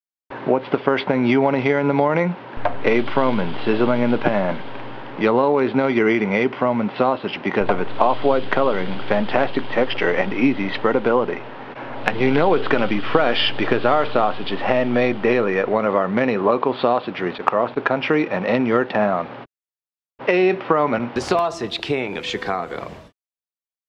Abe Froman Commercial 1
Abe Froman or as he is known to many "The sausage king of Chicago" put out many commercials during the early years of what is now  the largest sausage empire in the world. there was a small reference made to Abe Froman in the popular   American drama "Ferris bueller's day off". this commercial is old and barely recognizable these days,  compared to the hi-def cgi effects, and flashy advertising that the sausage king now uses.

anyone  who grew up in America when this commercial originally aired during Saturday morning cartoons will surly get a warm feeling from seeing this again.